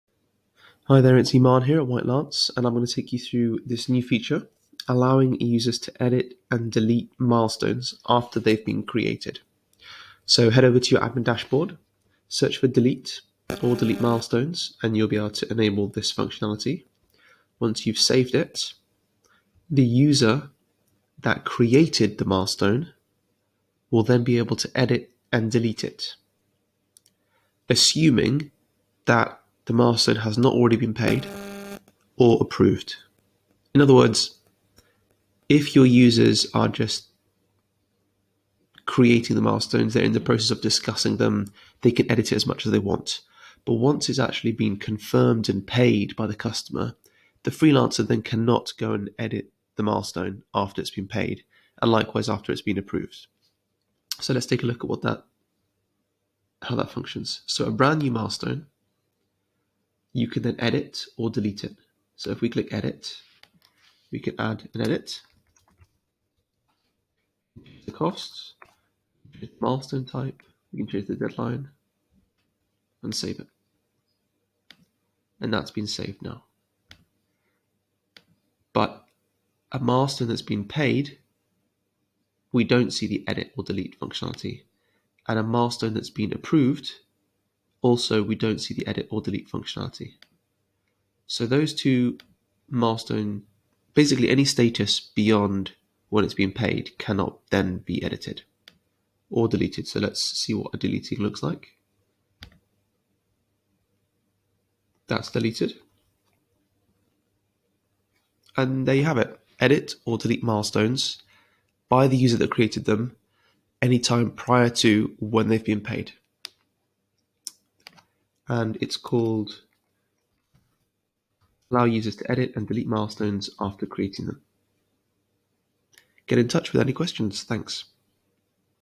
Allow users to edit and delete milestones
This video guide explains how the user that created a milestone can edit it before it has been approved or paid. Within a contract there can be several milestones, each with its own start & end date.

In the admin dashboard this feature is enabled by going to 'Settings' and selecting  'Allow users to edit and delete milestones after creating them?'